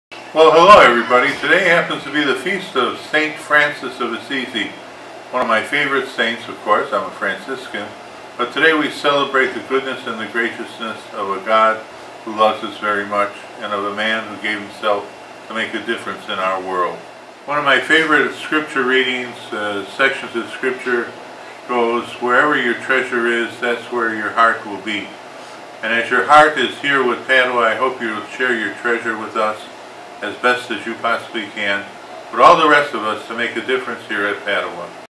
Fr. Allan Wishes You a Happy Feast Day!
It's October 4th, the Feast Day of St. Francis of Assisi! We invite you to be part of Bruins Big Give -- Padua's 3rd Annual Day of Giving! Join us to help make a difference in the lives of our Bruins and Future Bruins!